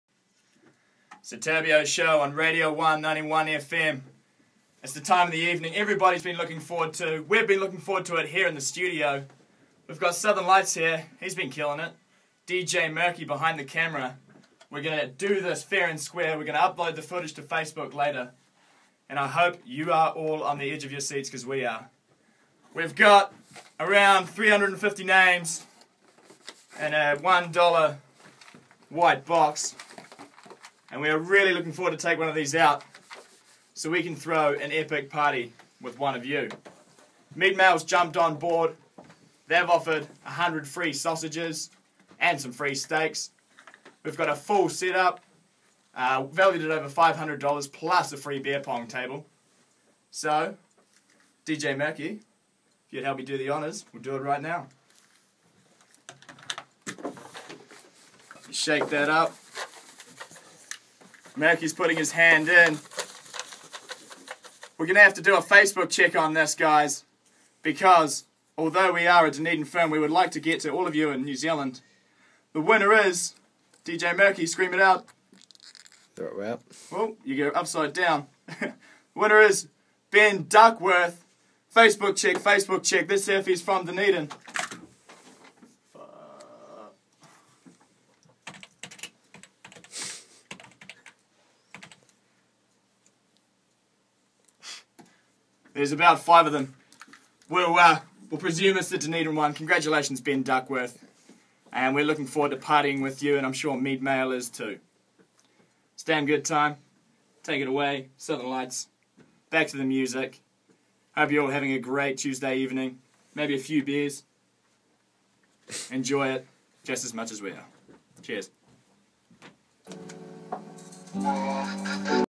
Turbio Sound & Meatmail Prize Winner
We ran a promotion with Meatmail to give away a "Project X" Party Package + Ultimate BBQ including:

- Speakers
- Lighting
- A Professional DJ
- Massive BBQ pack, with 100 high quality pre-cooked sausages and some prime steaks.
- A Custom Made Turbio Sound Beer Pong Table for you to keep

Watch the video to find out who won.